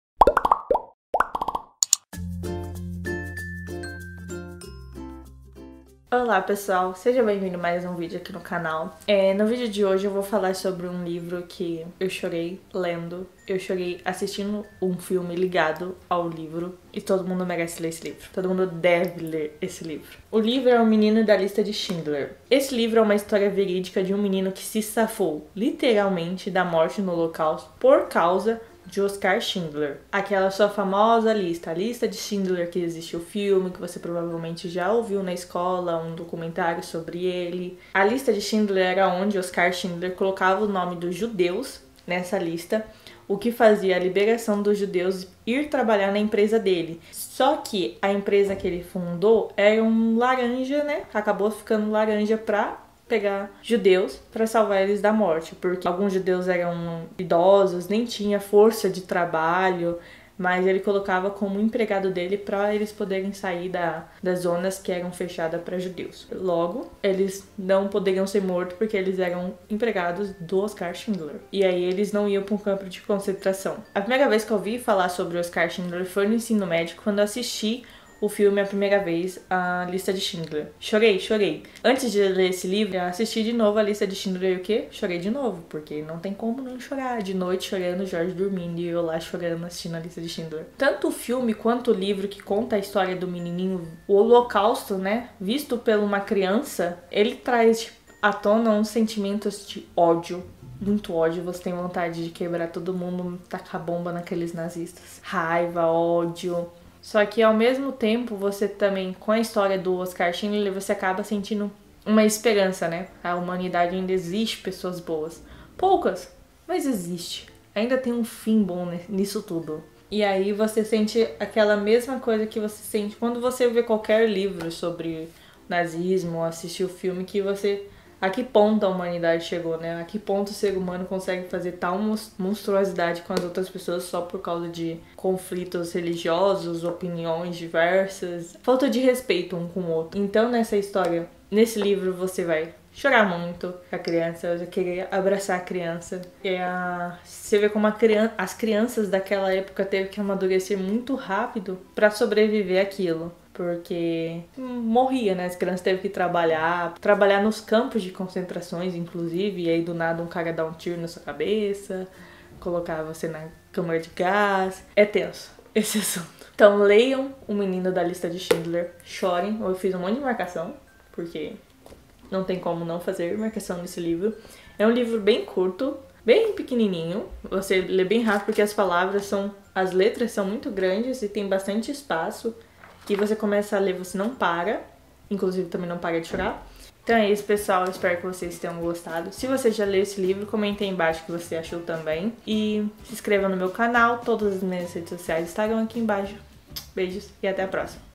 Resenha: "O Menino da Lista de Schindler" de Leon Leyson | Ariany Lippi
Olá amores, hoje eu trago a resenha de um livro que me fez chorar litros, que é "O Menino da Lista de Schindler" de Leon Leyson.